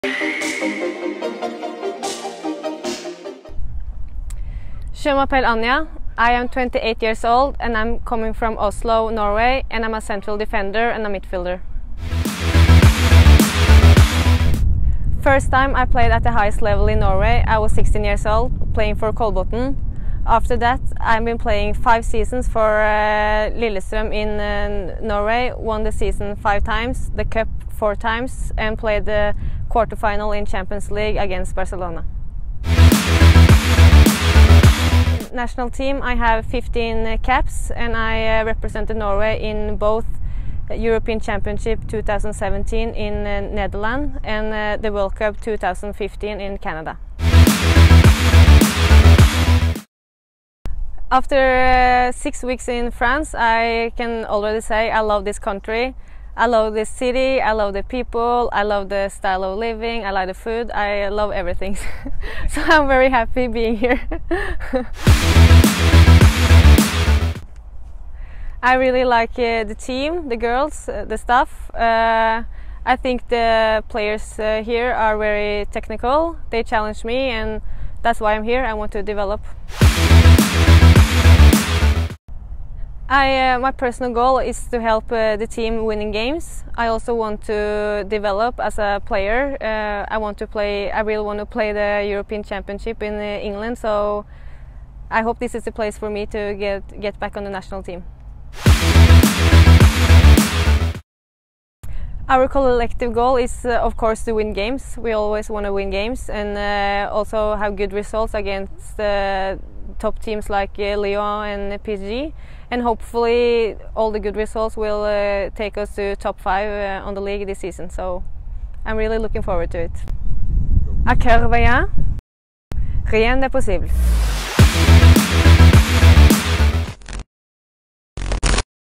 INTERVIEW - Anja Sønstevold (Défenseure D1F)
Entretien avec la nouvelle défenseure du FC Fleury 91 Anja Sønstevold. L'internationale Norvégienne est arrivée cet été en provenance de Lillestrøm.